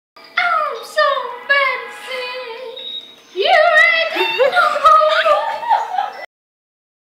Vanesa dzied
Pirms treniņa mums Vanesa nodziedāja dziesmiņu